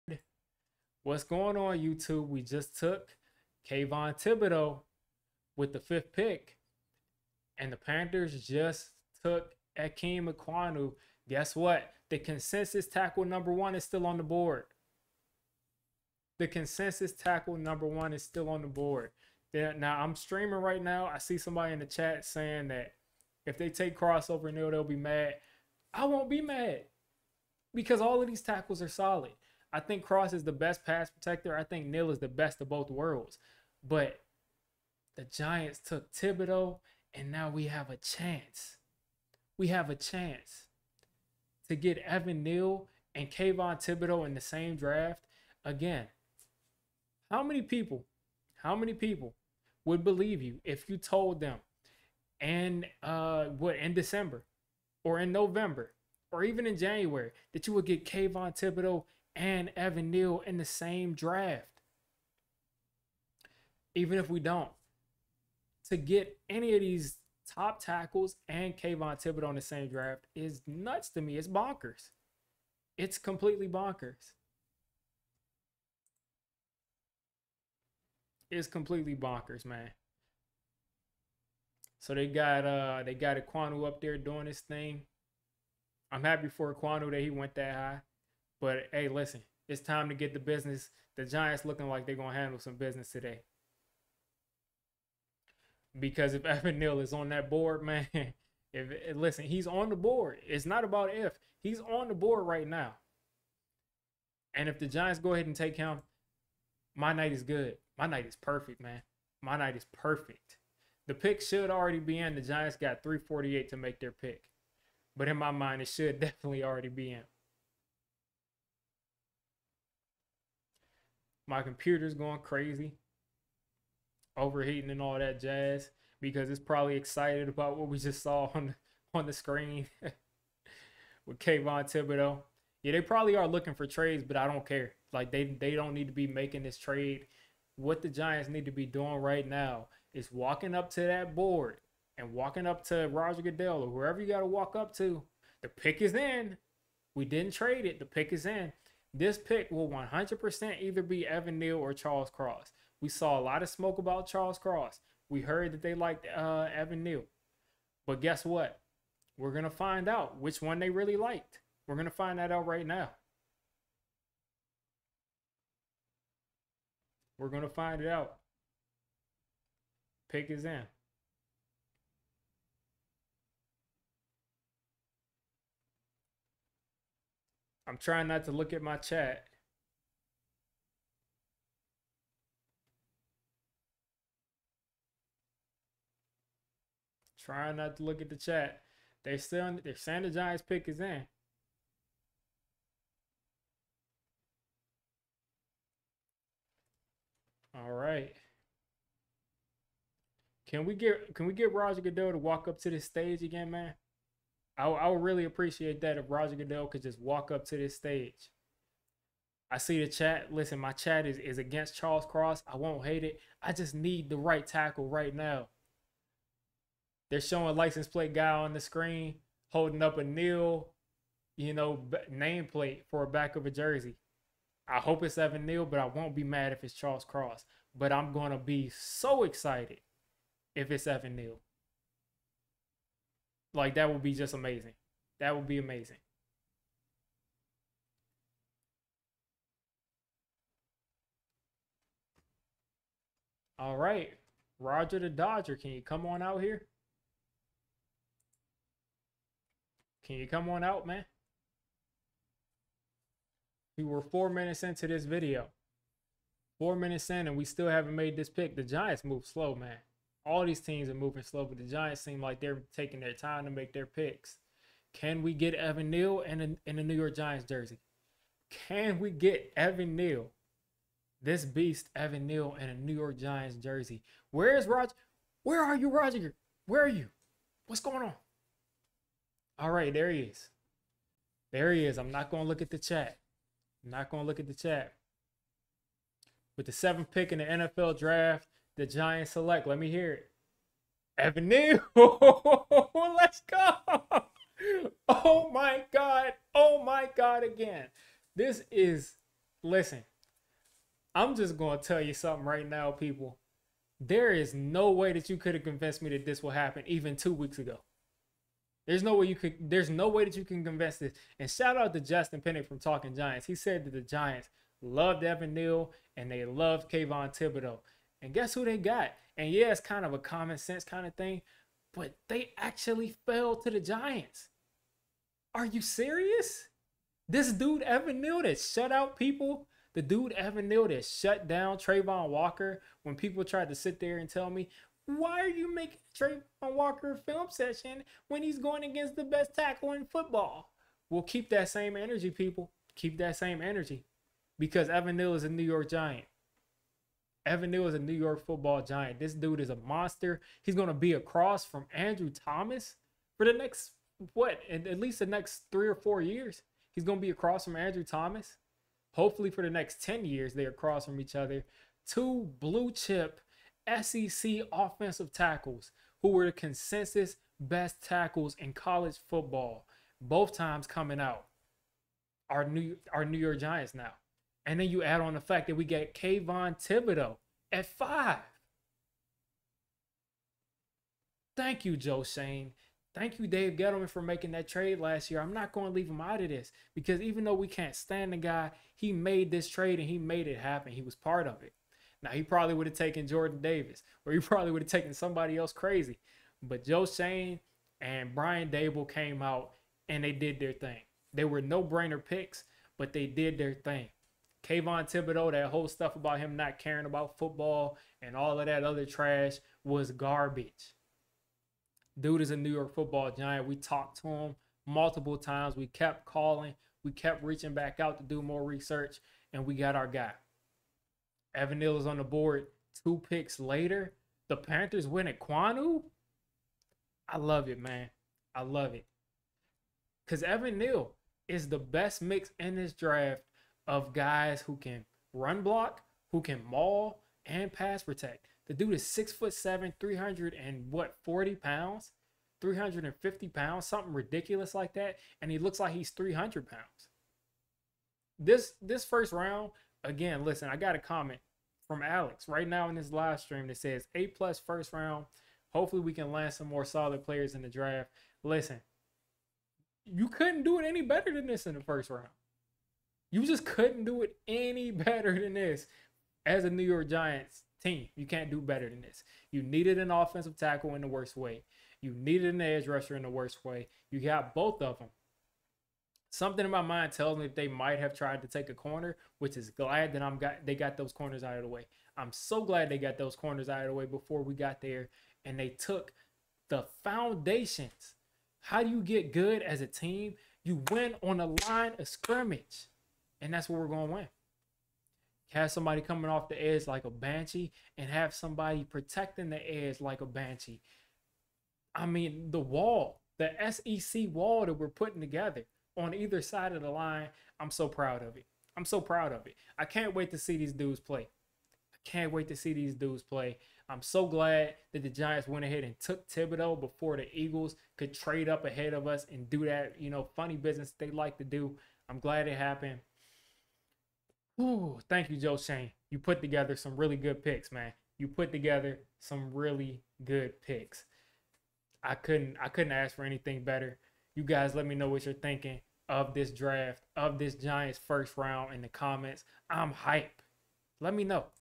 New York Giants Fan Reacts to The Giants Drafting Evan Neal | Joe Schoen is the Truth
Joe Schoen took Evan Neal as the New York Giants right tackle of the future right after taking Kayvon Thibodeaux. I'm on cloud 9